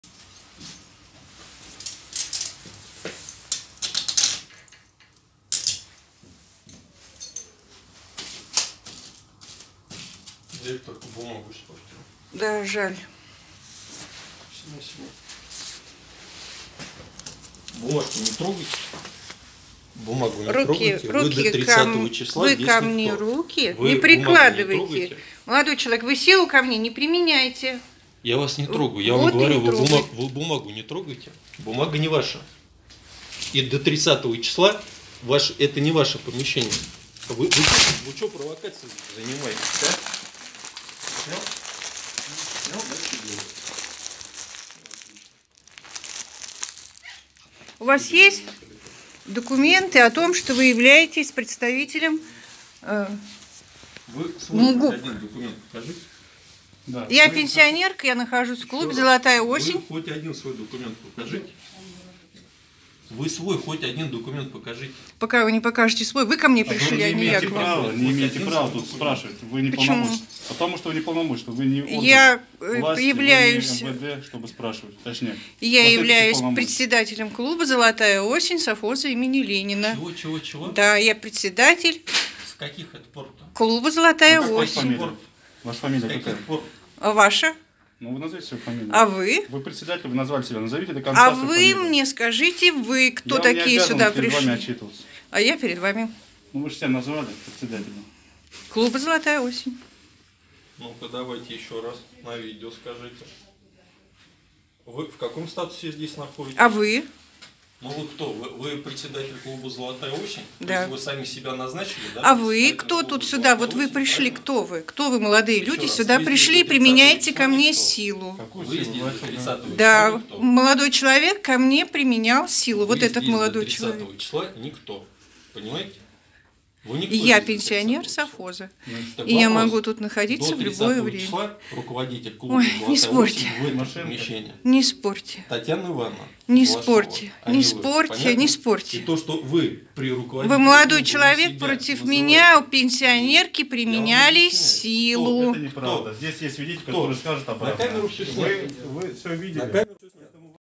Титушки напали на ветерана в Совхозе имени Ленина
Титушки боевого братства ворвались в помещения клуба ветеранов. Когда пенсионерка Совхоза имени Ленина, руководитель клуба «Золотая осень» Наталья Павловна Зинина попыталась пресечь хулиганские действия к ней была применена физическая сила.